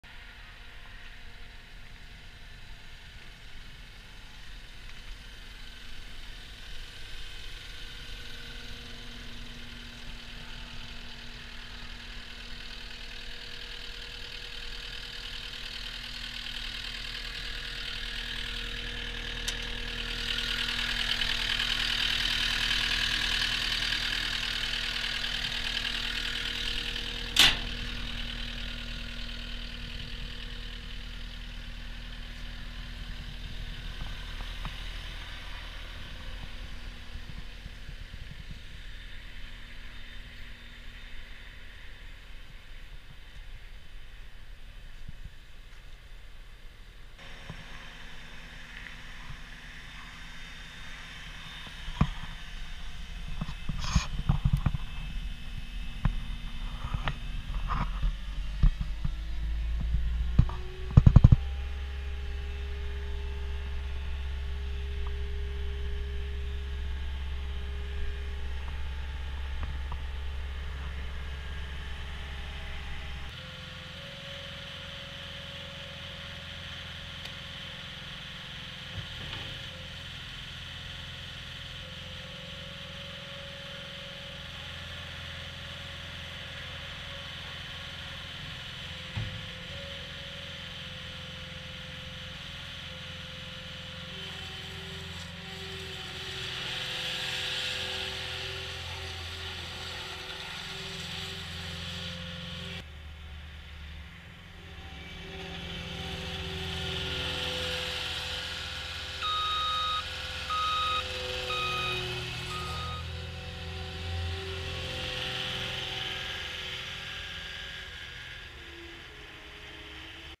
2015 CAT 242D SKID STEER LOADER
Walk around and operation of CAT 242D s/n:DZT01994.on Tue Feb 07 10:06:35 CST 2017